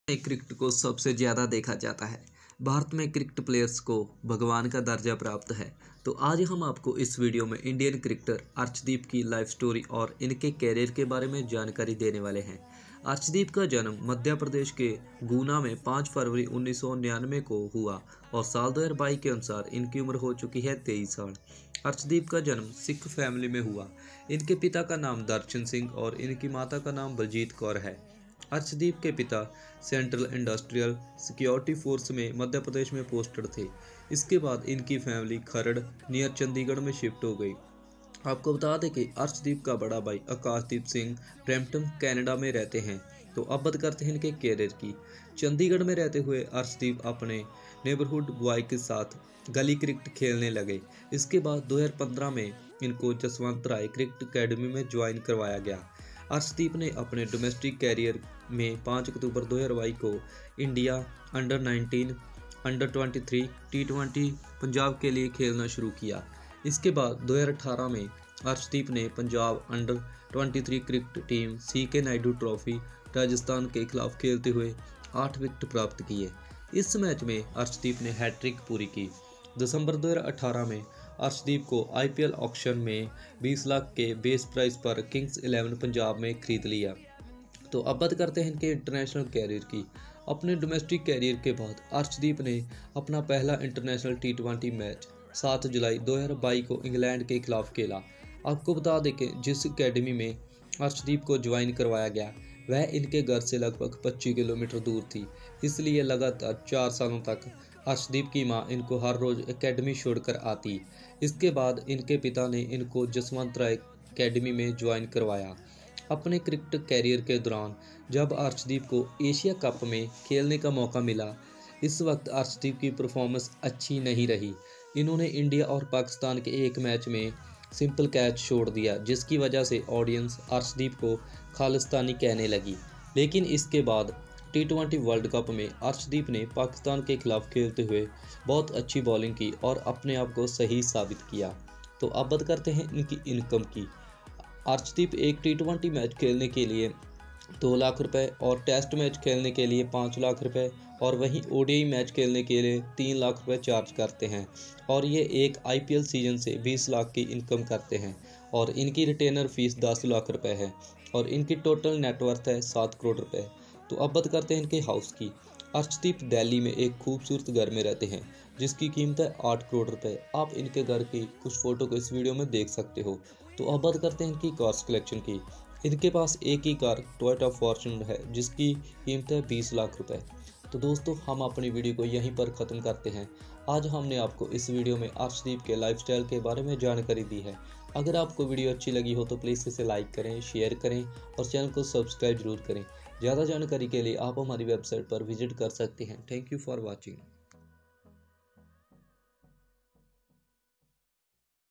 Arshdeep Singh (Cricketer) Biography, Career, Income, House, Family, Wife & Cars Collection
Check Written Biography of Arshdeep Singh